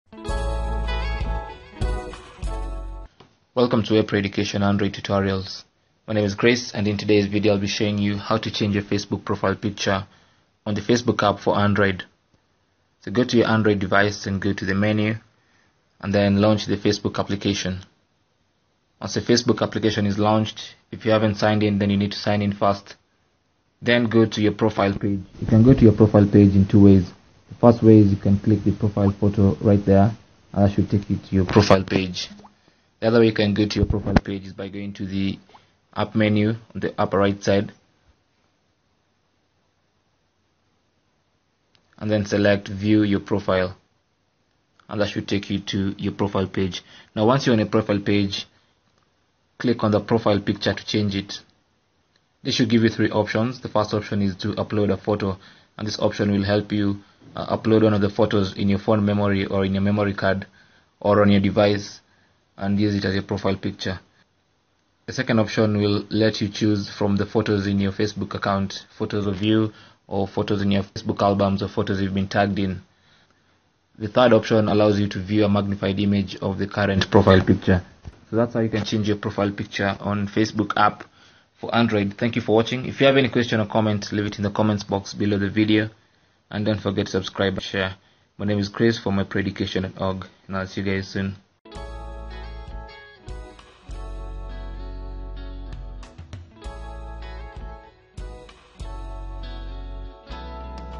How To Change Profile Picture On Facebook [Android]
Learn how to quickly change your Facebook profile picture using the Facebook app on your Android device.

Just like on the desktop interface,  changing your Facebook profile picture is easy on the Android Facebook app.

How To Change Facebook Profile Picture On Android:

Launch the Facebook app and sign in if you haven't.
Go to your profile page by clicking on your profile picture or from the menu.
On your profile page,  touch on the profile picture.  Then edit your profile picture by either uploading a new picture or selecting one from your Facebook photos.